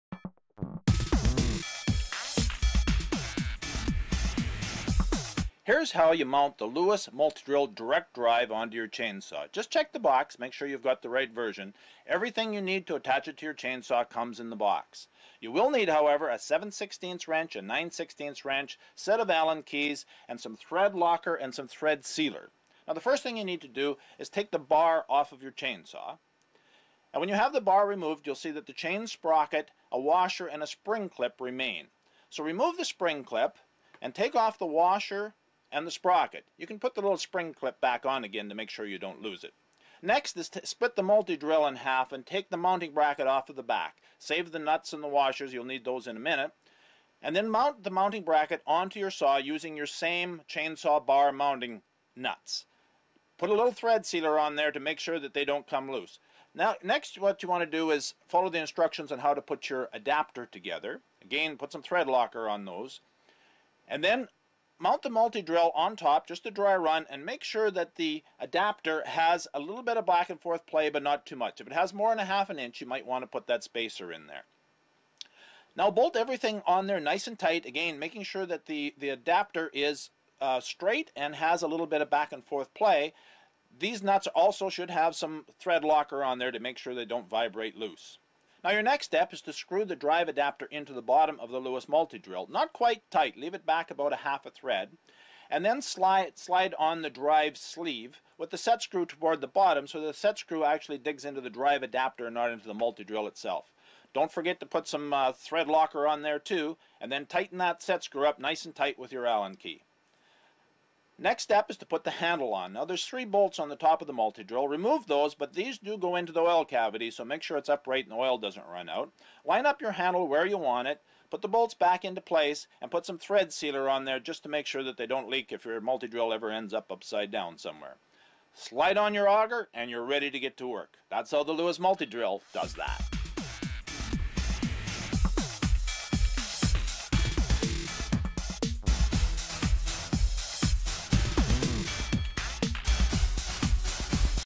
How to mount the Lewis MultiDrill Direct Drive Version to your Chainsaw.
This video gives you easy step by step instruction on how to mount your Direct Drive Lewis MultiDrill on to your chainsaw so you can drive Earth Augers, Ice Fishing Augers and Wood Drilling options. The Direct drive version requires a chainsaw with an outside sprocket. If your chainsaw has an inside sprocket then buy the Lewis Multidrill Universal Drive version which fits all chainsaws.